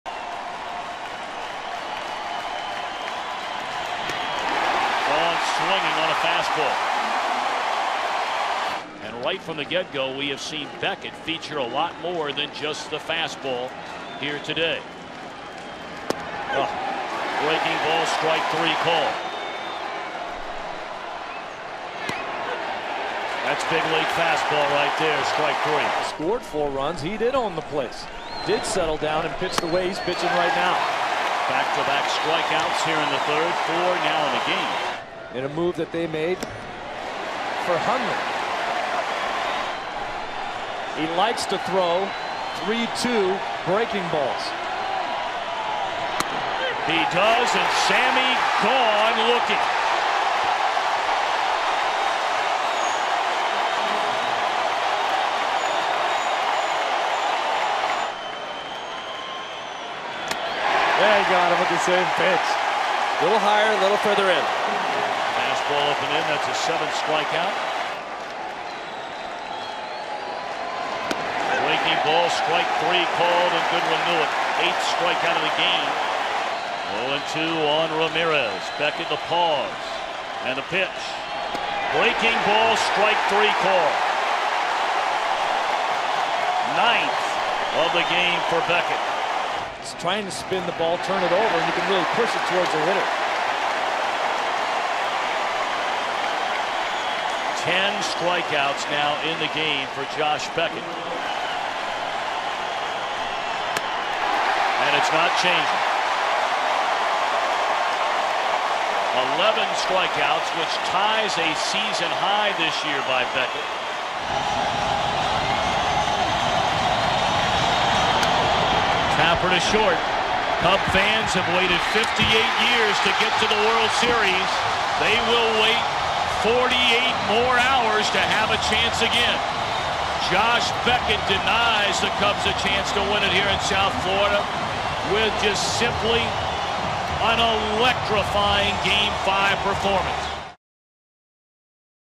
Beckett notches 11 strikeouts in '03 NLCS Gm5 shutout
10/12/03: Josh Beckett records 11 strikeouts in a masterful complete-game win over the Cubs in Game 5 of the 2003 NLCS